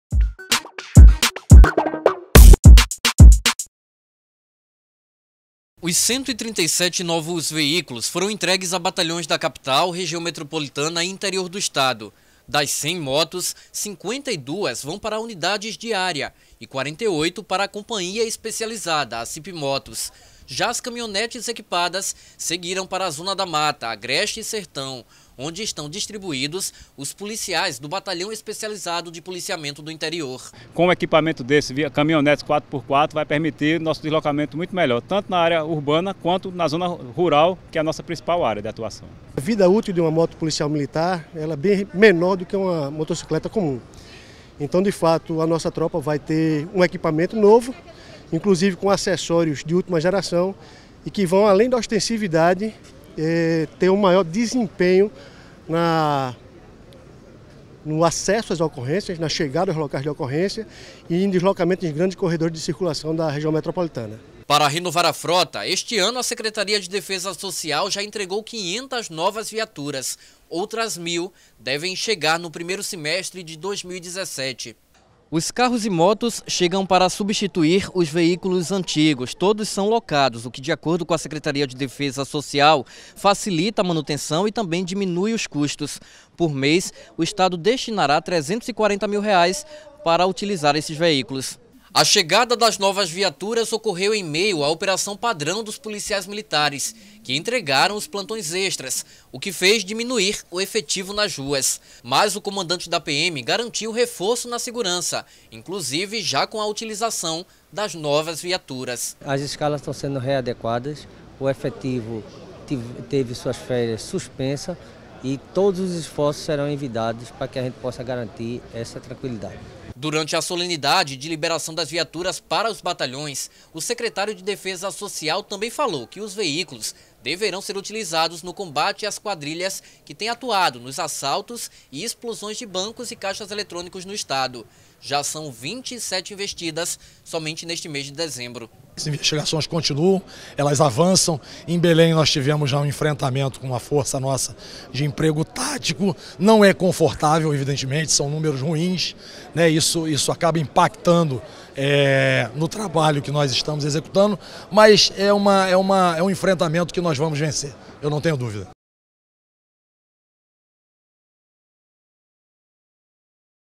Secretaria de Defesa Social reforça segurança com 137 viaturas
FIQUE POR DENTRO DE TUDO O QUE ACONTECE NO BRASIL E NO MUNDO. VEJA TAMBÉM: